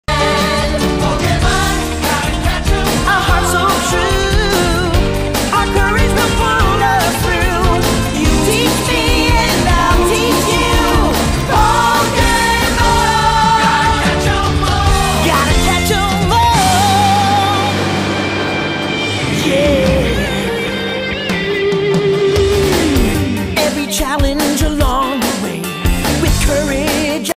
Was it REALLY worth £30? - Pokémon Costco Tin Opening #212
Let's take a look inside this grass tin from Costco. Was this set worth £30 total? I'll let you decide!